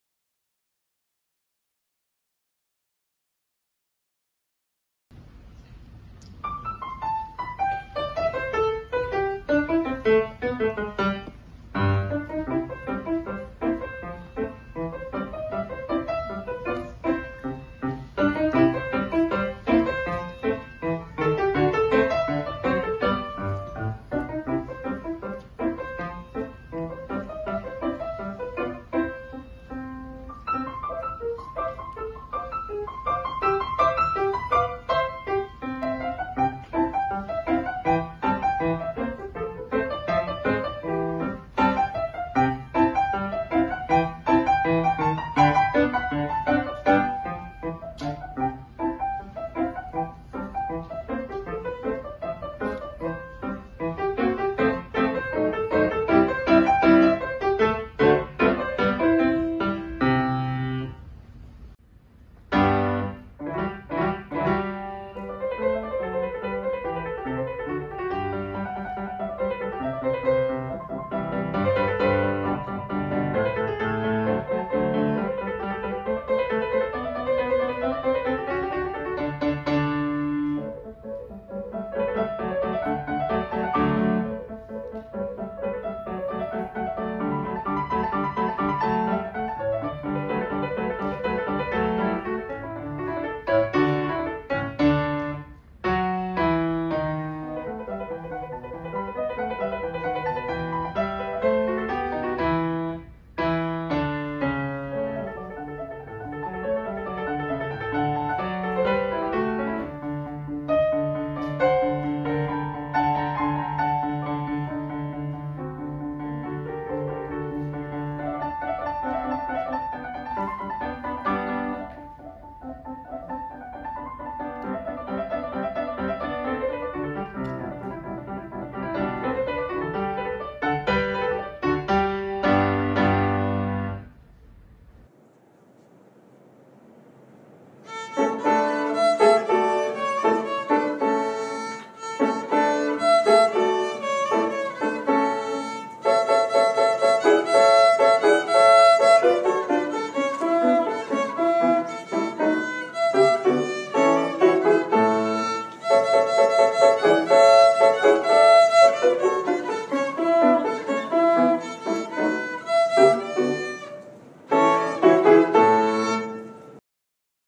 EuroAsia Final round 2023
Hanan - Second Prize in EuroAsia Final Round 2023
Yap Yong Guan - Pianist (Finalist)
Lucas Mak Yek Peng - Violinist (Finalist)